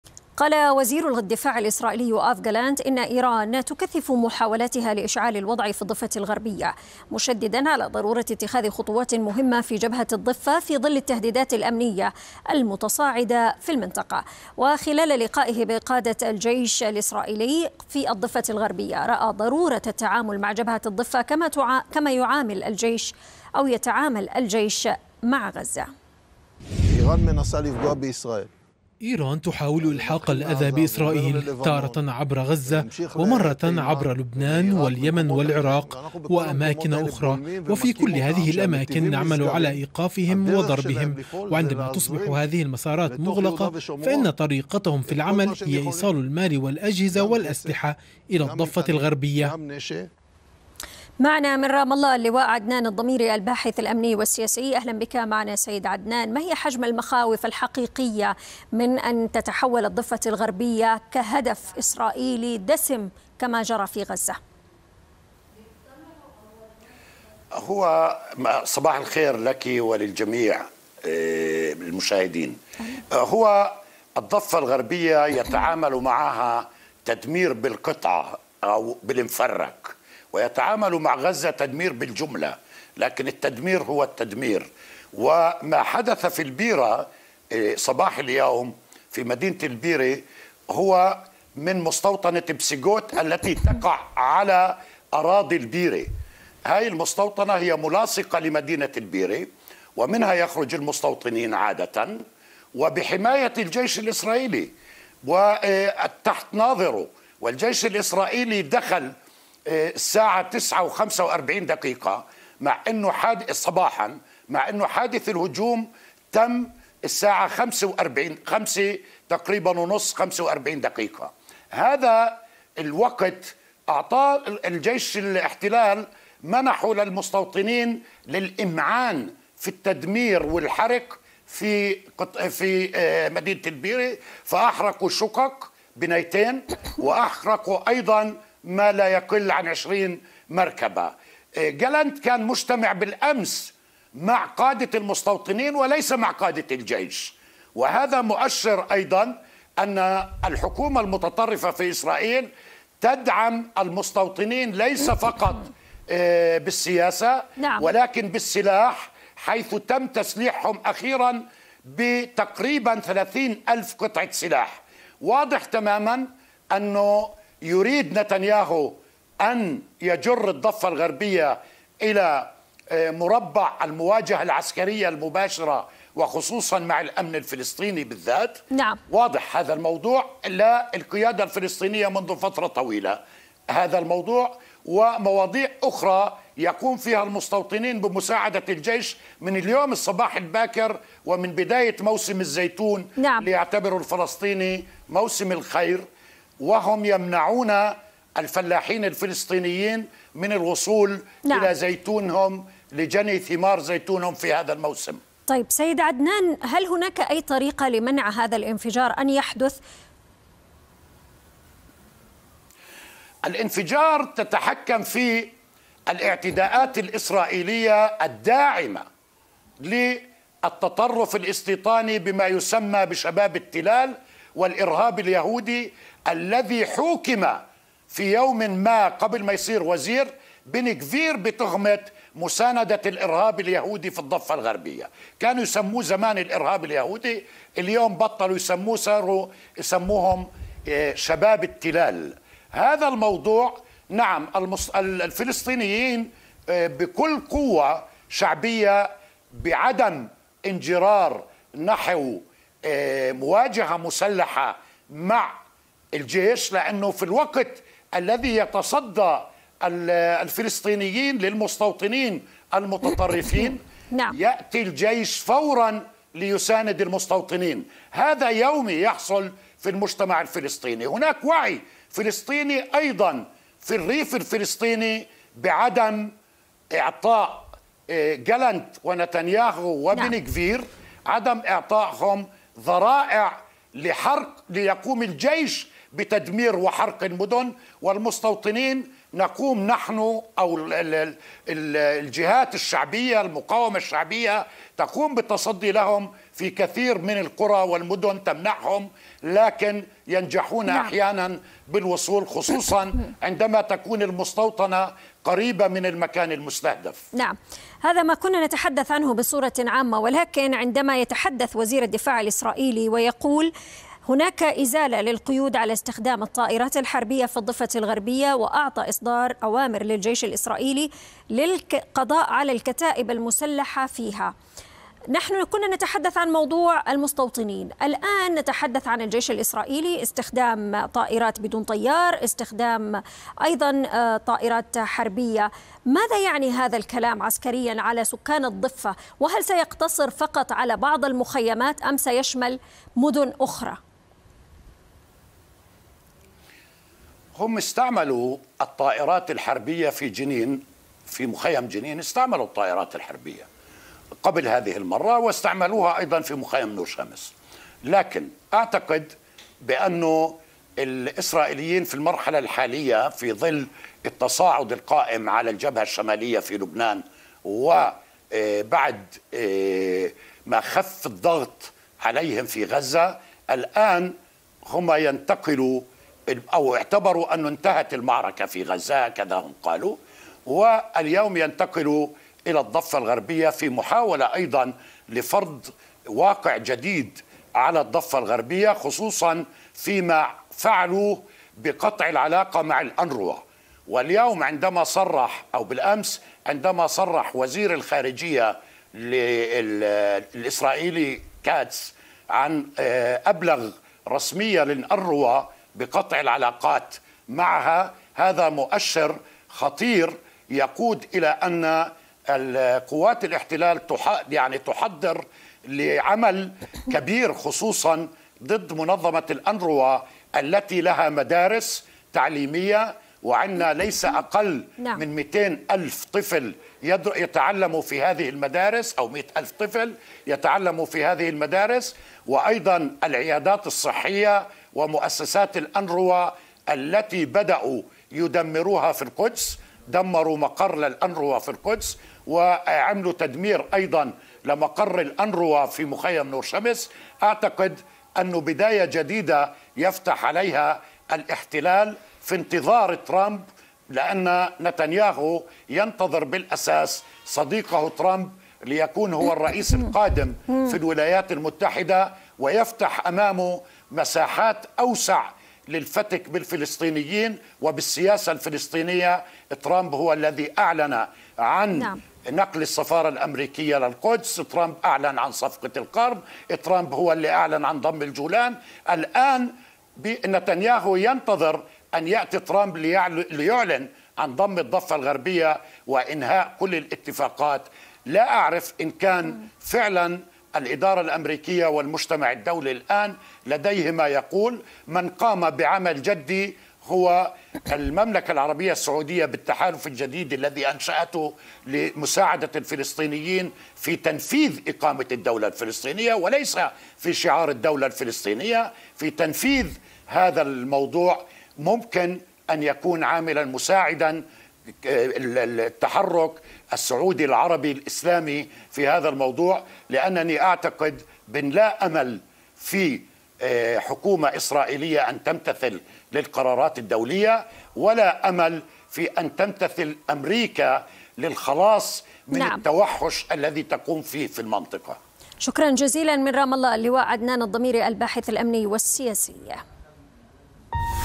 وزير الدفاع الإسرائيلي: يجب معاملة الضفة الغربية كغزة
غالانت يتهم إيران بتكثيف محاولاتها لإشعال الضفة الغربية التي يريد معاملتها كجبهة غزة

لمعرفة أحدث المستجدات.. عبر البث المباشر من هنا
وبإمكانكم متابعة الحدث على منصات التواصل